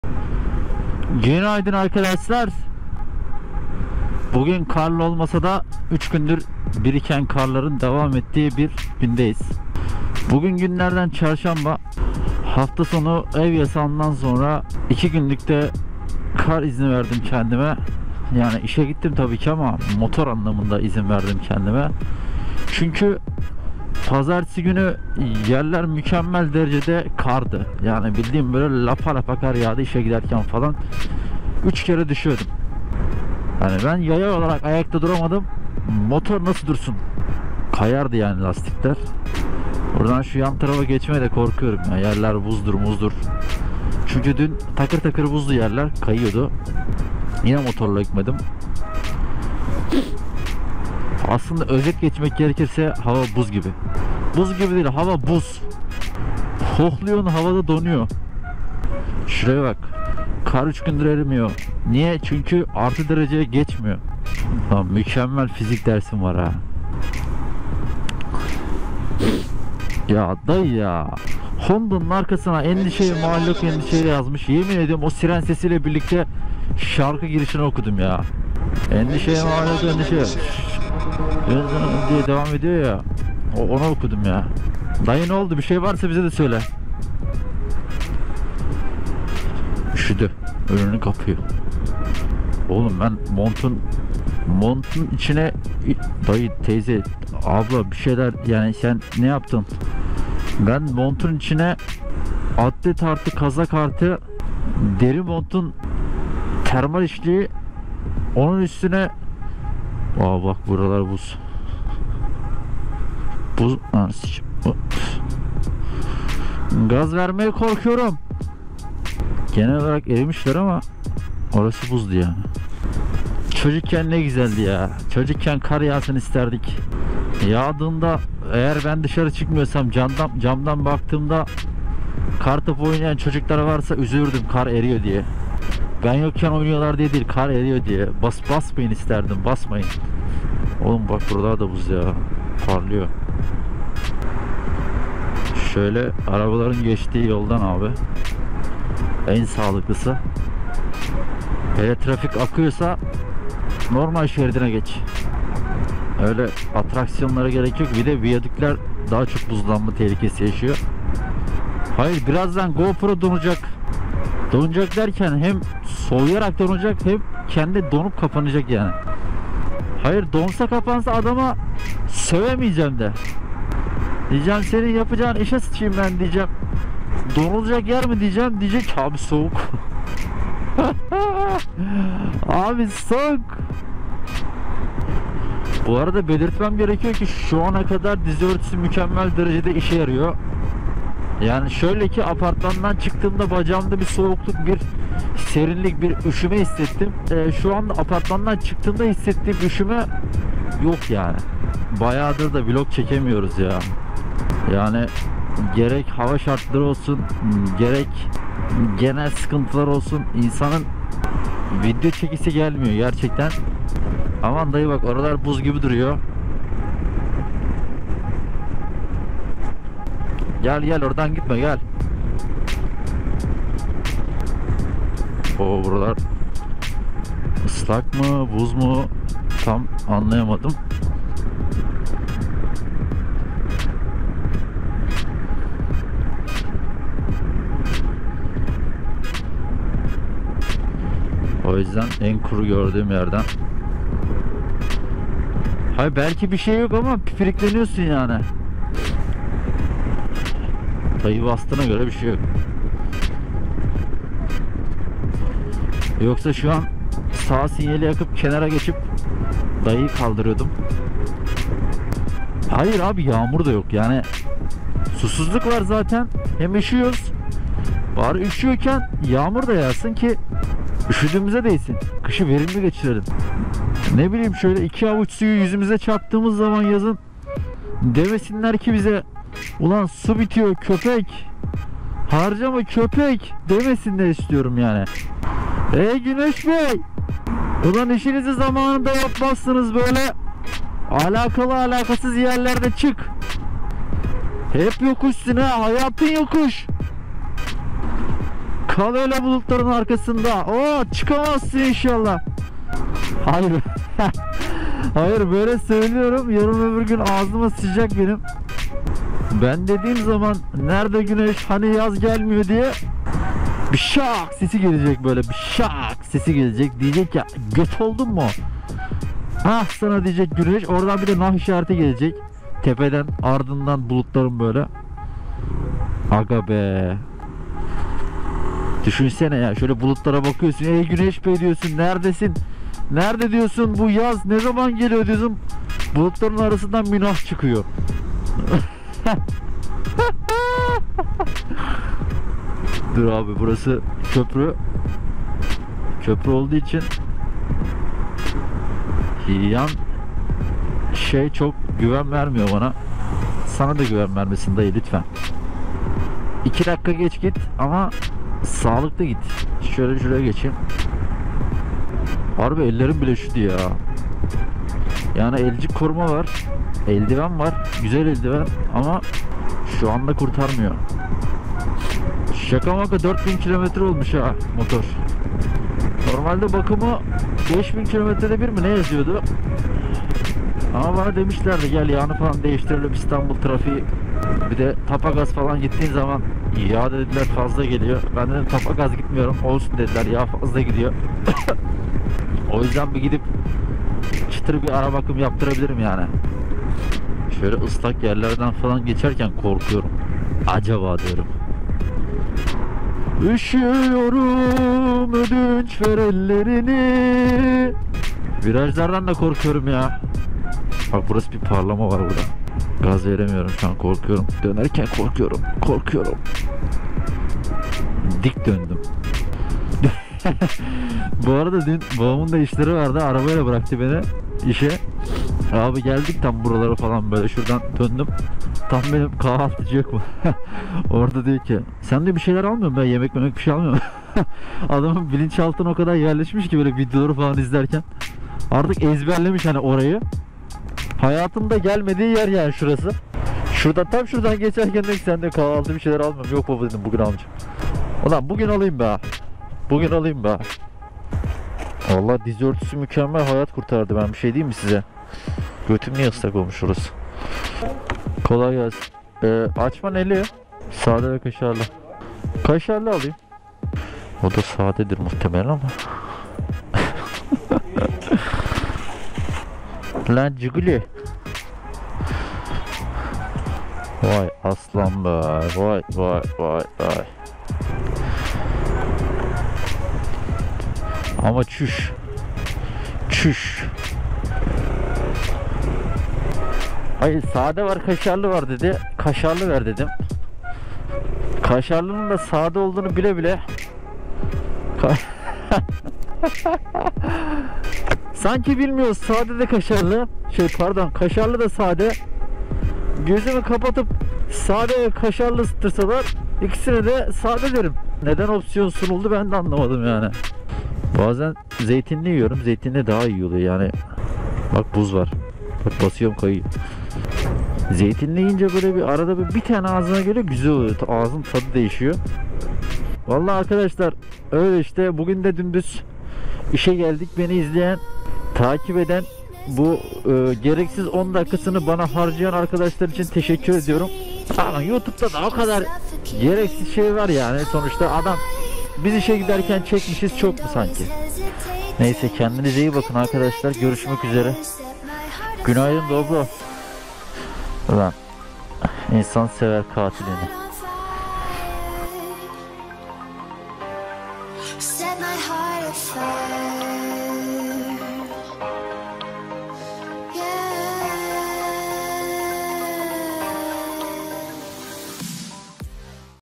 -5 Derecede Soğuk Bir İş Günü | GoygoySohbetlerSerisi
Merhaba Herkese,

İstanbul'un bembeyaza uyandığı bir haftadan bir vlog gelsin dedim. Malum şartların vloglarımızın arasını açtığı bir dönemde yeni video sizlerle :):))

Haydi öptüm, grş.